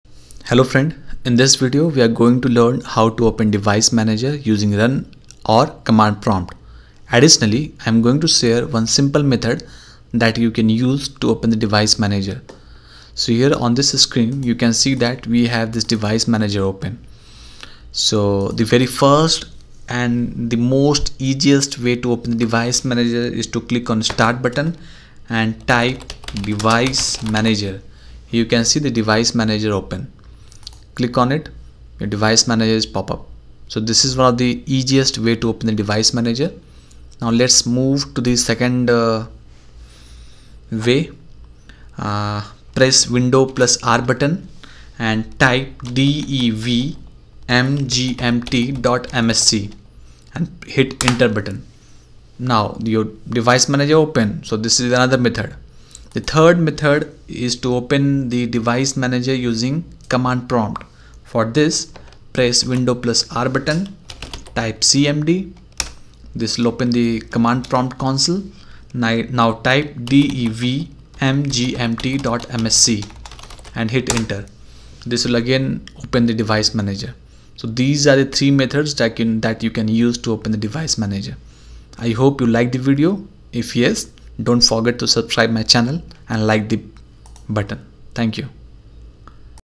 How to open Device Manager using Run or Command Prompt Shortcut
In this video I will guide how to open Device Manager using Run or Command prompt. Additionally, I have shared one shortcut method that you can use to launch Device Manager on your Windows 7, 8 or 10.